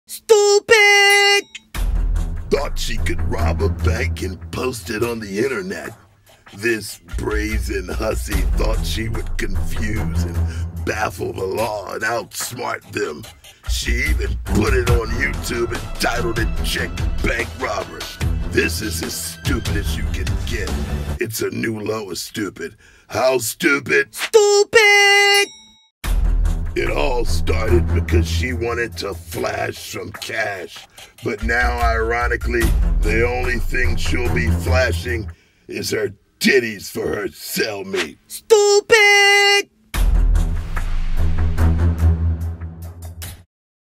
DUMBASS YOUTUBE BANK ROBBER!
This pretty young thang went gangsta & robbed a bank but then doubled her gangsta by actually posting it on YouTube!